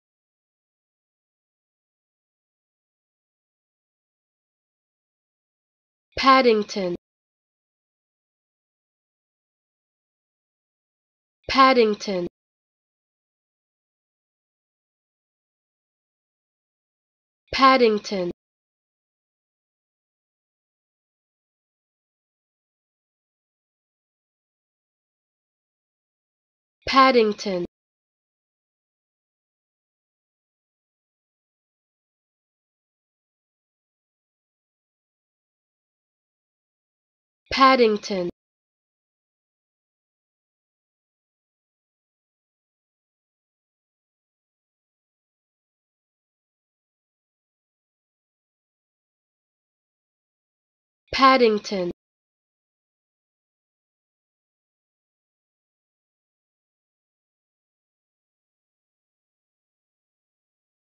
How To Pronounce Paddington
Learn how to say Paddington with EmmaSaying free pronunciation tutorials.